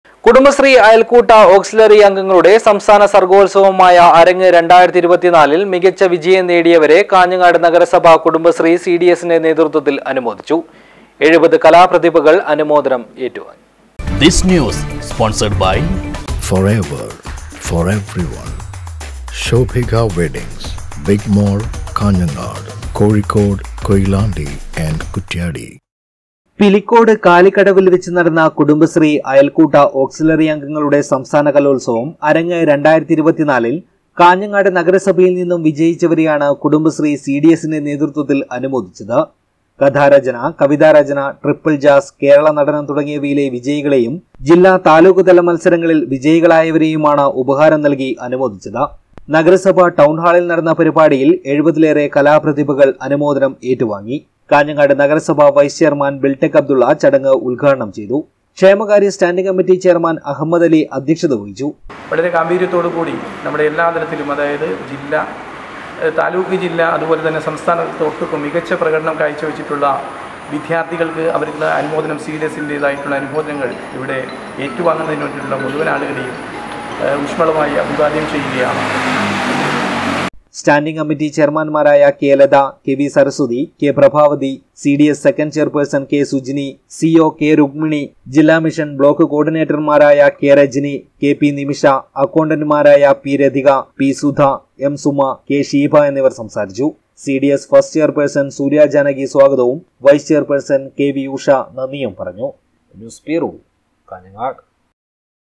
അരങ്ങ് 2024ല്‍ മികച്ച വിജയം നേടിയവരെ കുടുംബശ്രീ സി.ഡി.എസിന്റെ നേതൃത്വത്തില്‍ അനുമോദിച്ചു.
കുടുംബശ്രീ അയല്‍ക്കൂട്ട ഓക്‌സിലറി അംഗങ്ങളുടെ സംസ്ഥാന സര്‍ഗോത്സവമായ അരങ്ങ് 2024ല്‍ മികച്ച വിജയം നേടി യവരെ കാഞ്ഞങ്ങാട് നഗരസഭ കുടുംബശ്രീ സി.ഡി.എസിന്റെ നേതൃത്വത്തില്‍ അനുമോദിച്ചു. എഴുപത് കലാപ്രതിഭകള്‍ അനുമോദനം ഏറ്റ് വാങ്ങി.